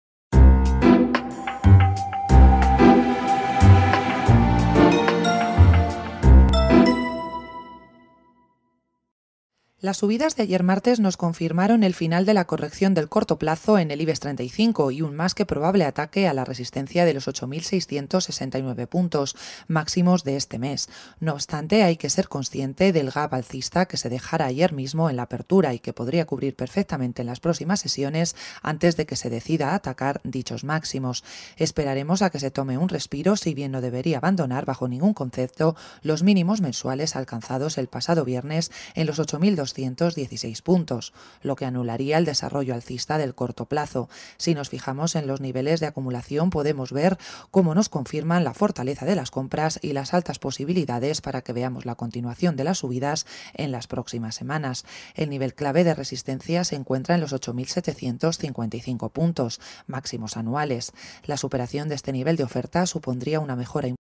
Análisis Técnico Ibex35
Esperaremos a que se tome un respiro si bien no debería abandonar bajo ningún concepto los mínimos mensuales alcanzados el pasado viernes en los 8.216 puntos.